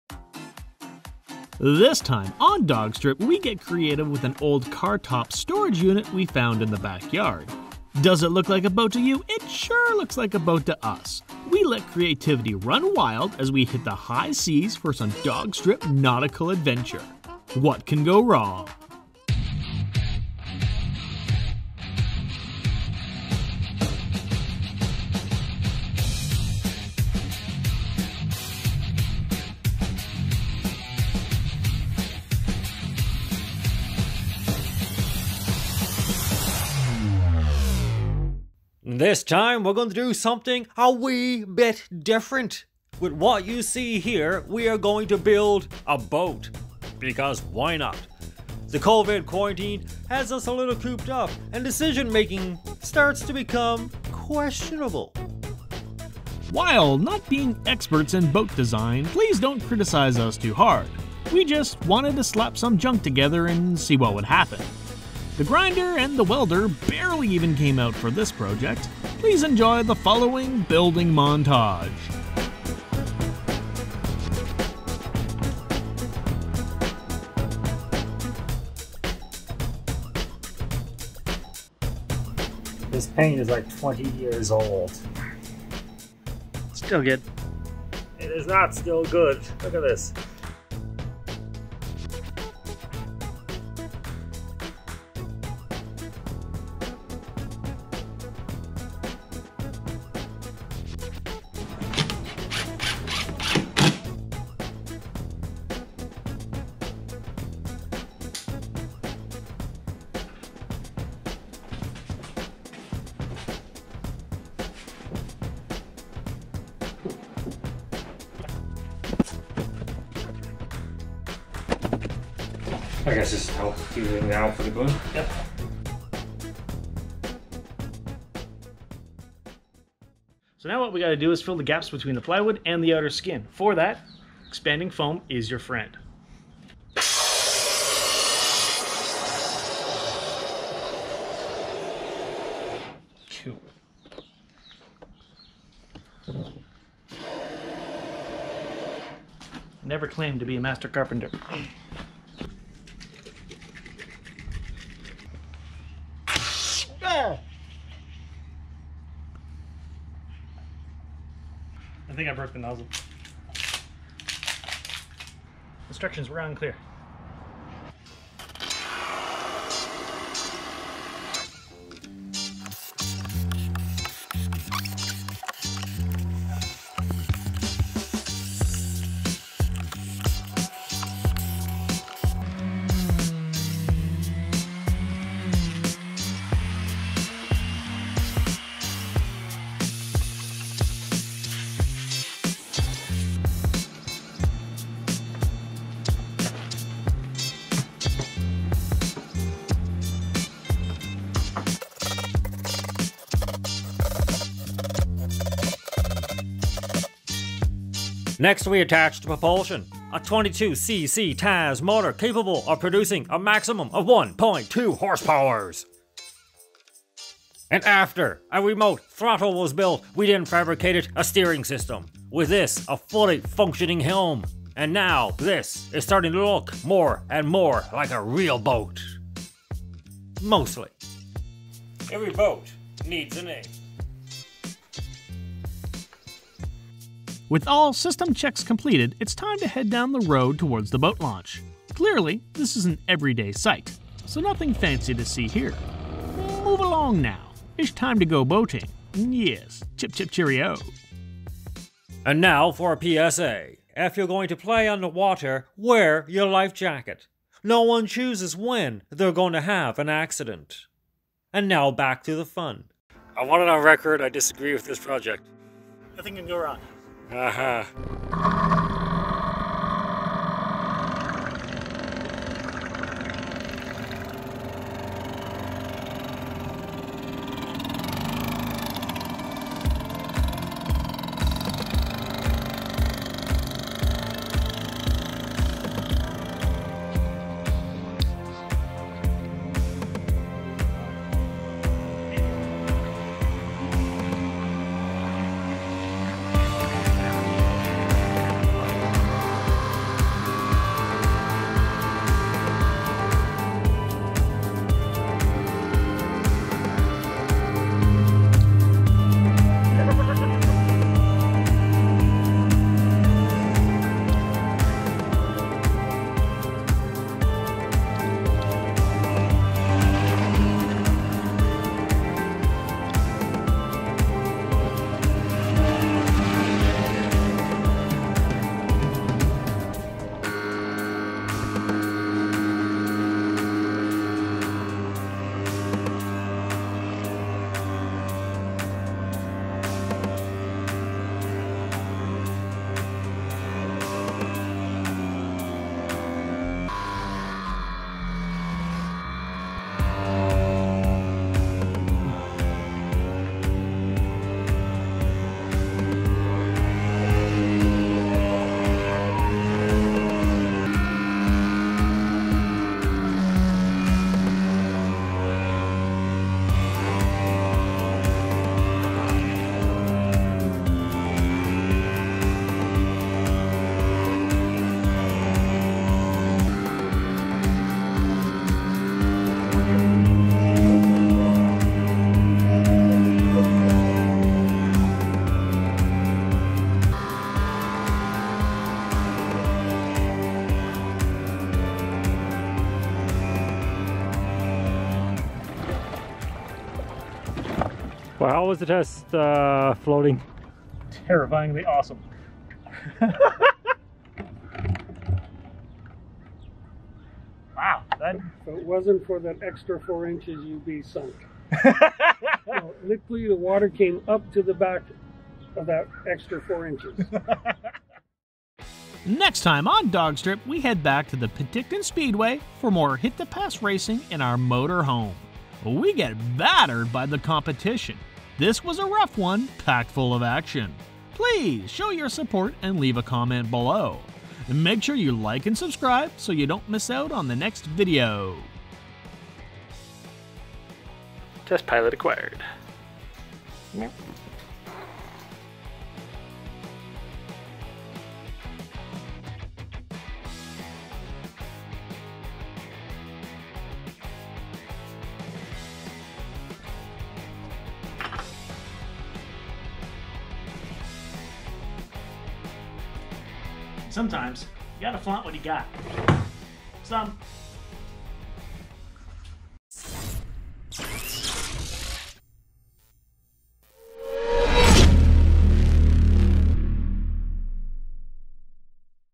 DawgStrip - CarTopper - Mini Boat
We build a boat out of a cartopper storage unit we found in the back yard. This project was on t he ToDo list for a while but due to the quarantine we had the time to do it.